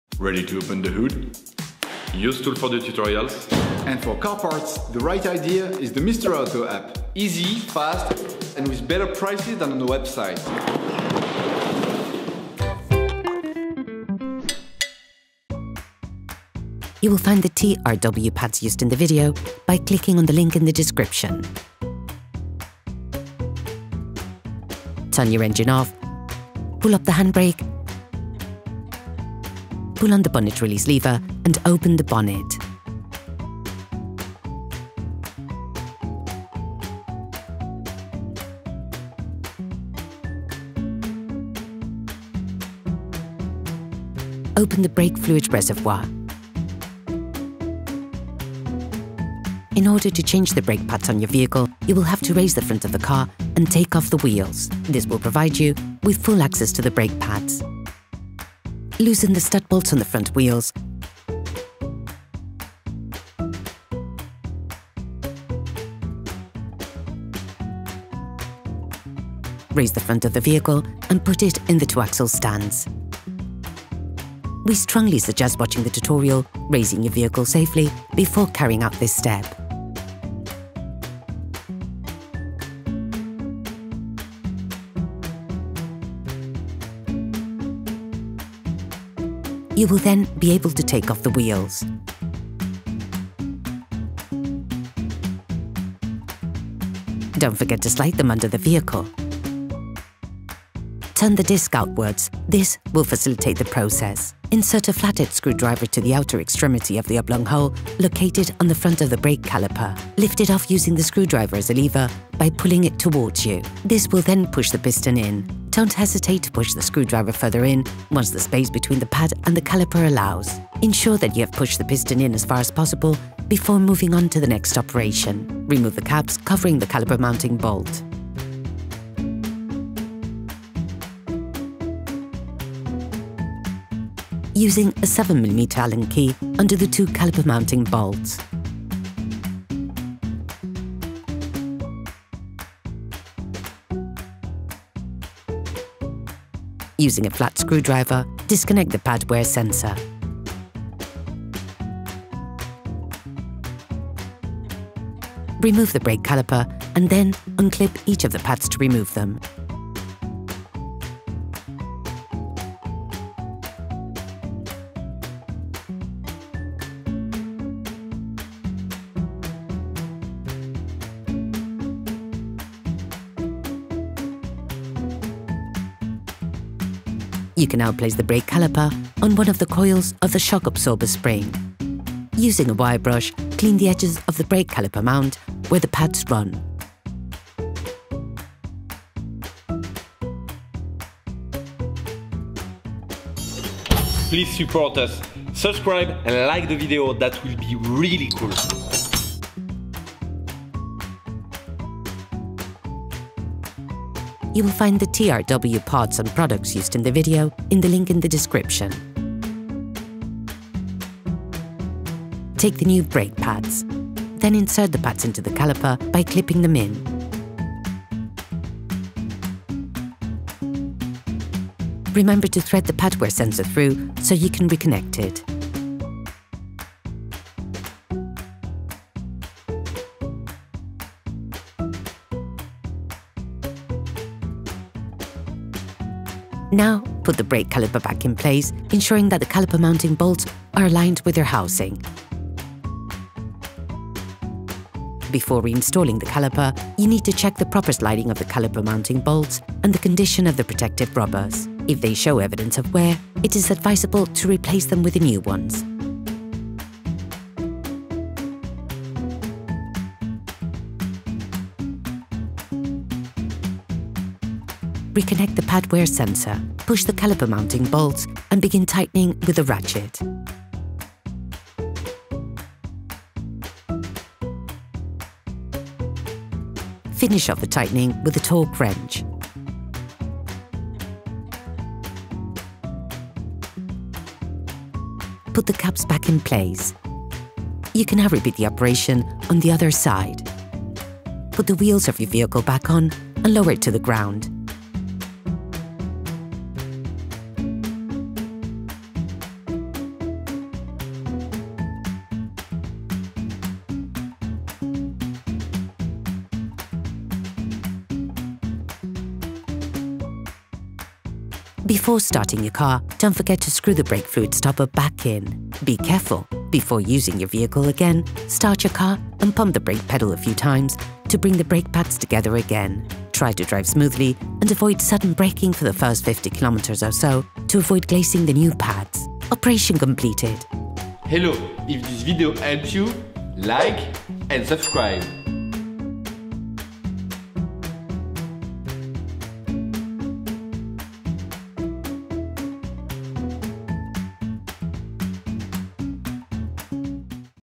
How to replace the front brake pads Golf mk7 🚗
Front brake pads on a Volkswagen Golf 7.

Needed tools:
-💣🤖🏆The complete toolbox recommended by UseTool ES to repair your Golf 7

- Big flat head screwdriver
- Socket wrench
- Flat head screwdriver
- 7mm Allen key
- Wheel lug wrench
- Torque wrench
- 7mm Allen socket
- Wire brush

🔝💪Safely lift your Golf 7 with the hydraulic jack and axle stands recommended by UseTool EN 💪🔝

- Hydraulic jack
- Wooden peg
- Axle stand

🚗📢💡 RESOURCES 💡📢🚗

Usetool's warnings
- Once the operation is completed, you'll need pump slowly on the brake pedal a few times until feeling a strong resistance from the pedal. The aim being to bring the brake pads closer to one other.

- Drive smoothly and avoid sudden braking for the first 50 kilometres or so to avoid glazing the new pads.

- Make sure to check the stability of your vehicle before starting the operation.

- Don't burn yourself on the pads if you've just gone for a drive as braking creates friction and long-lasting heat.

Usetool's infos
- What do we mean by "glazing new brake pads"?

- Break pads are made of a material that needs to be run-in when new. If this procedure is not done properly, when braking too hard, the material overheats and hardens. Consequently, the brakes become less effective.

0:00 Intro
0:15 Remove the pads
3:01 Replace the pads
5:11 Tips